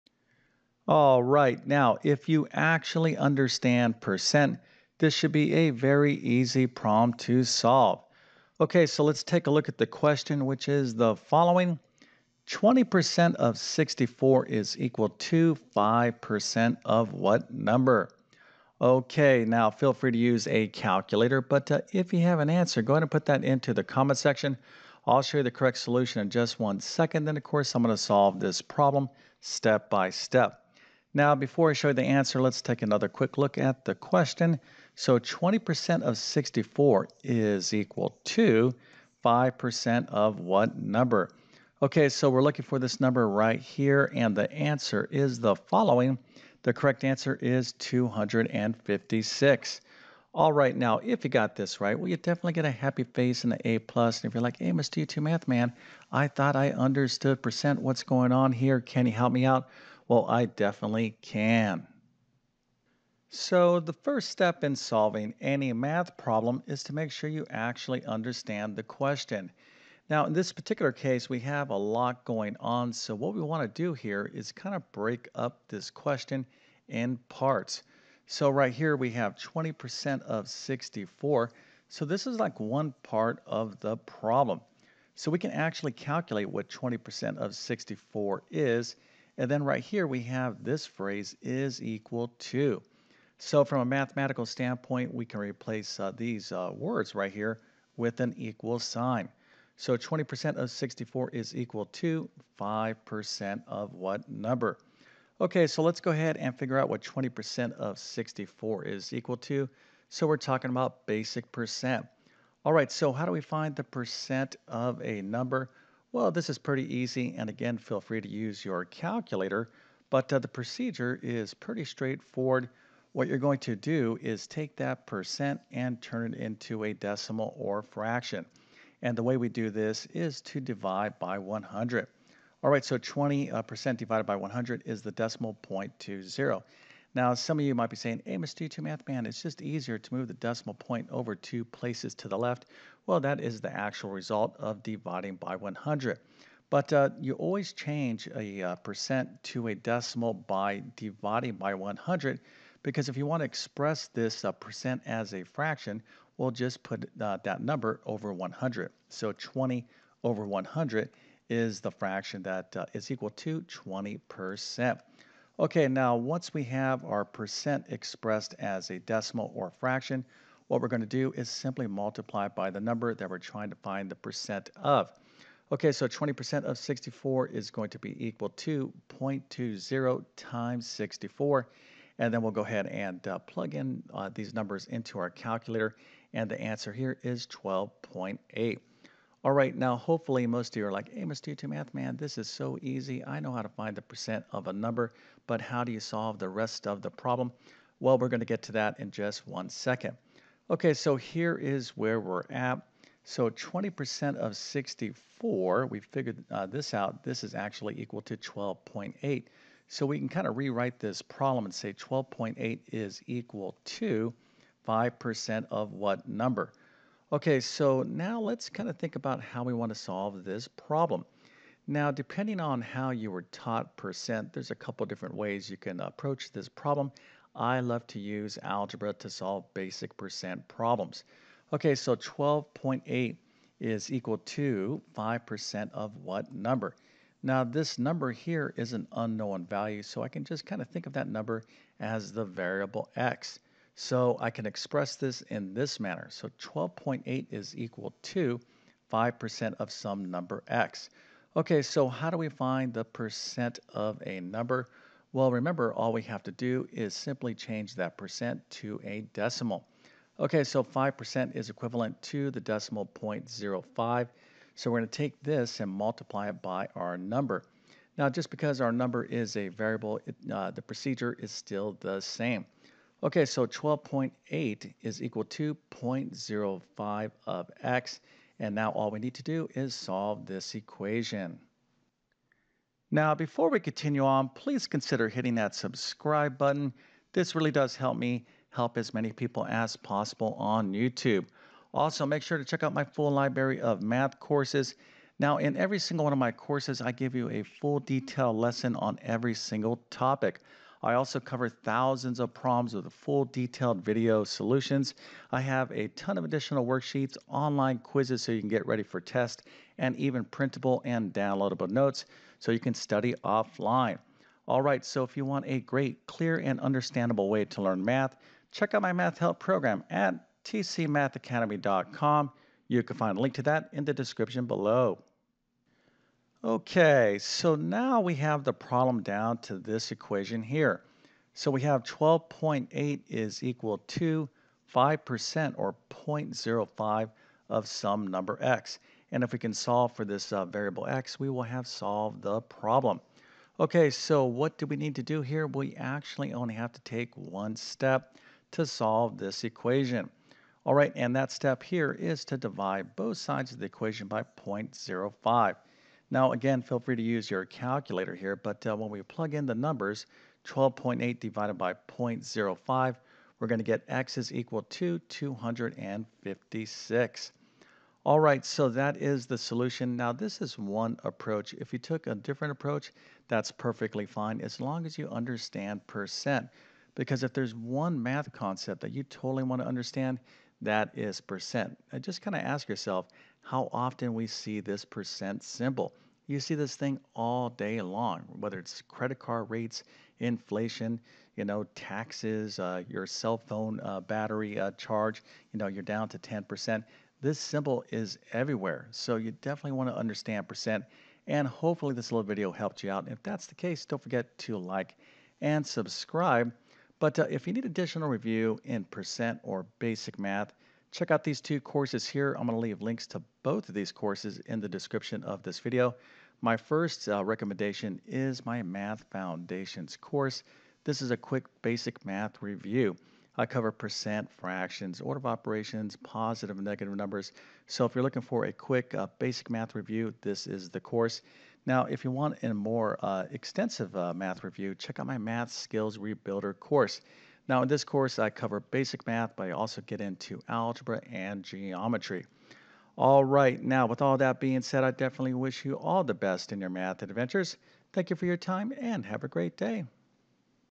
20% of 64 is equal to 5 percent of what number? MANY can’t solve this PERCENT problem!
How to solve a percent math problem.

• HOMESCHOOL MATH
• COLLEGE MATH